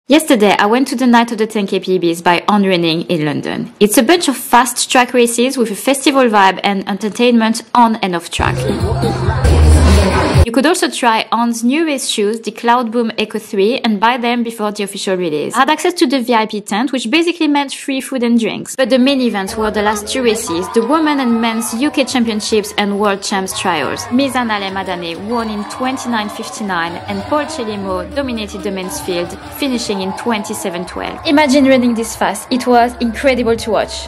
Night of the 10,000m PBs 2023 by On Running | Paul Chelimo & Mizan Alem Adane | Epic Track Races
Yesterday I went to the Night of the 10,000m PBS by On Running in London. It’s a bunch of fast track races with a festival vibe and entertainment on and off track.

You could try On’s new race shoe, the Cloudboom Echo 3 and buy them before official release.
I had access to the VIP tent which basically meant free food and drinks!

The main events were the last 2 races, the women and men UK championships and World champs trials.
Mizan Alem Adane won in 29:59! Paul Chelimo dominated the men’s field, finishing in 27:12

Imagine running this fast!! It was an incredible event!